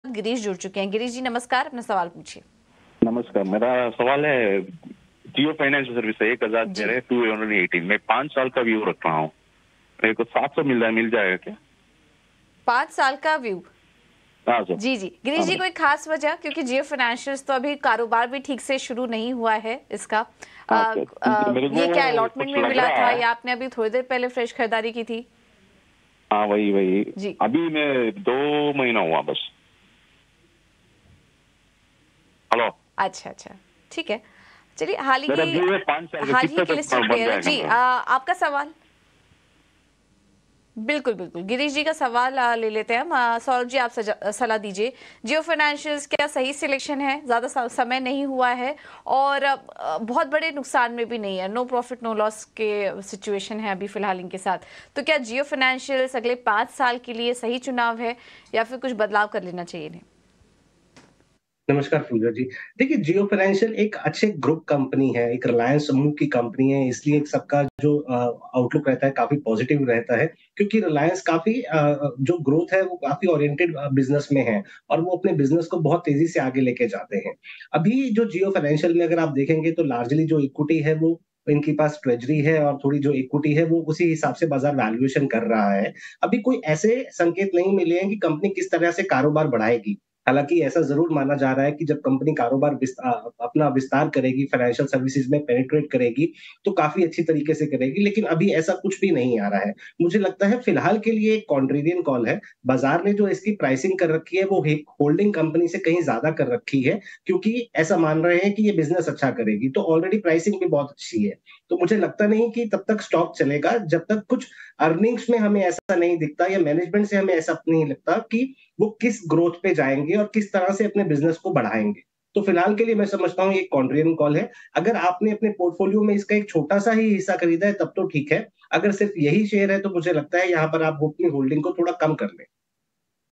Jio Financial के Share में Expert से जानिए निवेश की रणनीति, जानिए किस Level पर करें Buy or Sell
Jio Financial  में जानें निवेश को लेकर क्या है Experts की राय। जानें किन levels पर करनी चाहिए आपको खरीदारी और क्या है Target Price और जानिए किस Strategy के साथ इस Stock में पैसा कमाया जा सकता है देखें वीडियो।